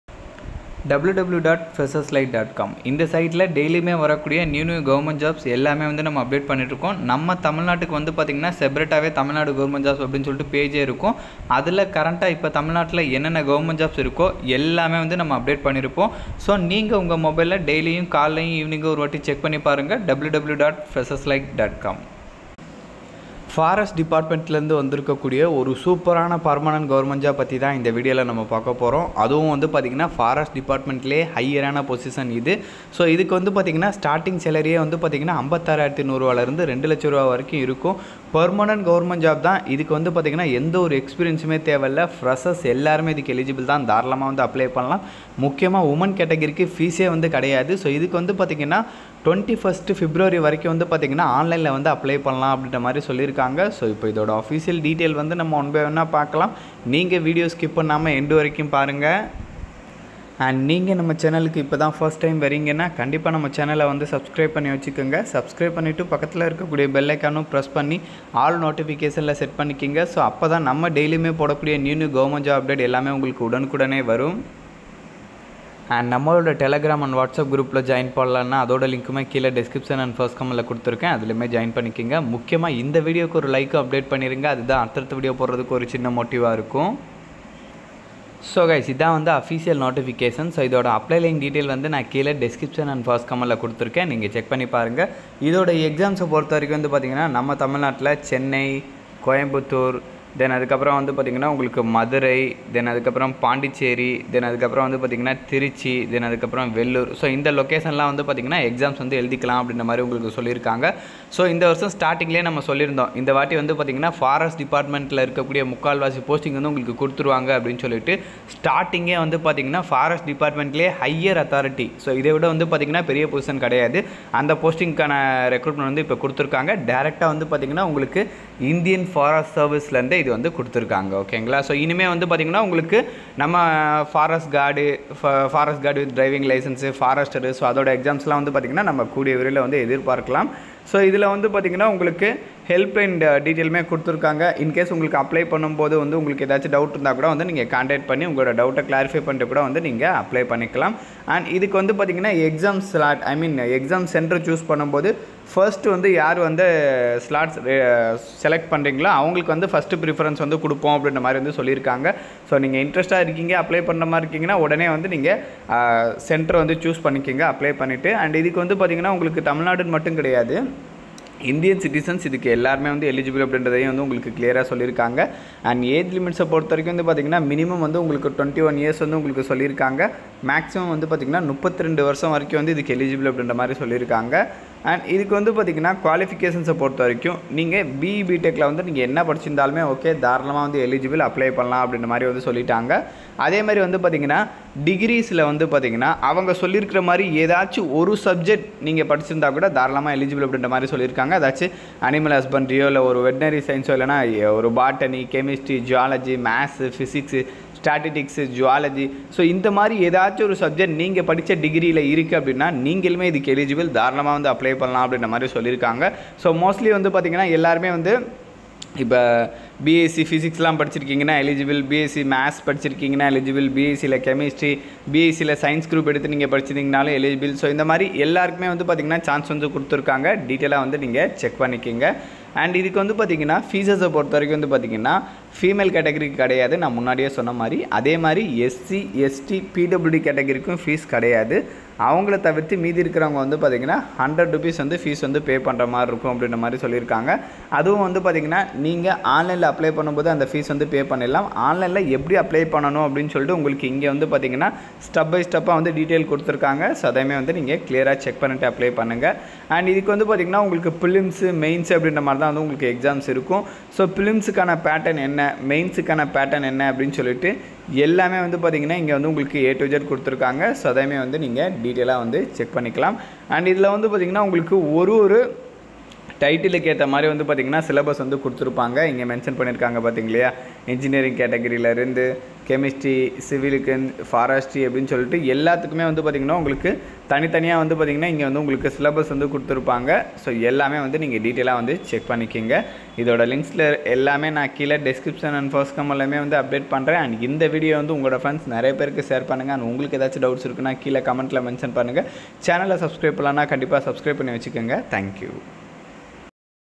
🔥 Forest Officer Notification 2023 |TN Forest Recruitment 2023| Permanent Government Jobs 2023 Tamil
இந்த வேலைக்கு விண்ணப்பிக்கும் கடைசி தேதி முடிந்துவிட்டது மேலும் தற்போது வந்திருக்கும் புதிய வேலைவாய்ப்பு பற்றி தெரிந்து கொள்ள நம் சேனலில் புதிதாக போடப்பட்ட வீடியோவை பாருங்கள்